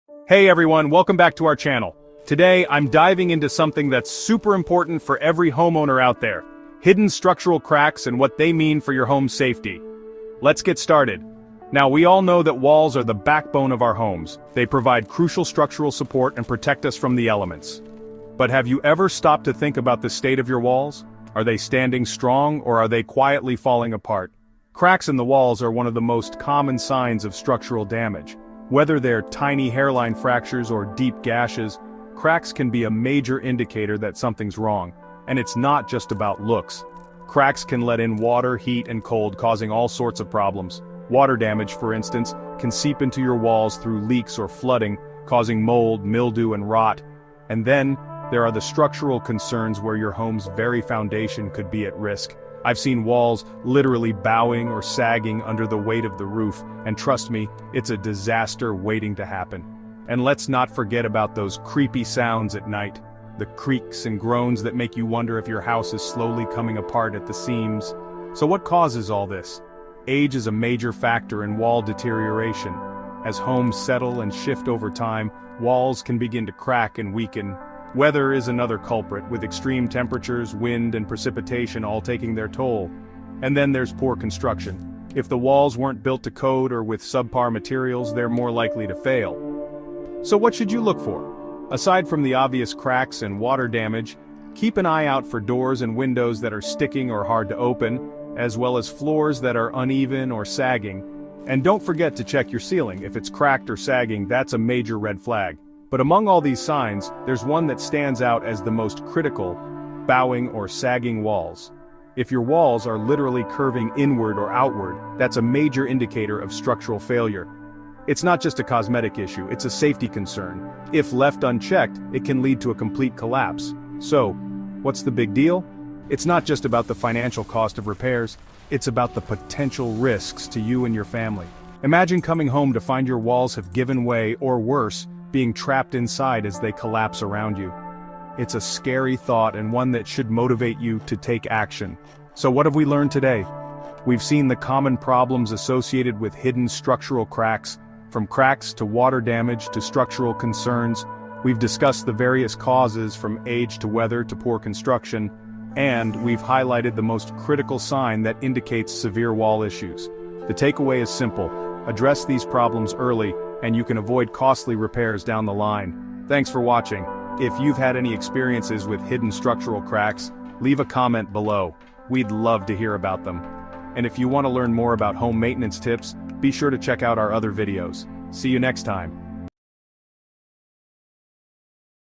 Is Your House Falling Apart? Check for Hidden Structural Cracks!
Are you concerned that your house might be falling apart? Don't ignore the warning signs! Hidden structural cracks can lead to serious safety issues and costly repairs if left unchecked. In this video, we'll show you how to check for fine cracks, hairline cracks, and other signs of structural damage in your home. From doors and windows to walls and ceilings, we'll cover the most common areas where cracks can appear. Learn how to monitor cracks and identify potential problems before they become major issues. Whether you're a homeowner or a structural engineer, this video is a must-watch for anyone concerned about house safety and building integrity. By the end of this video, you'll know exactly what to look for and how to take action to ensure your home remains safe and secure for years to come.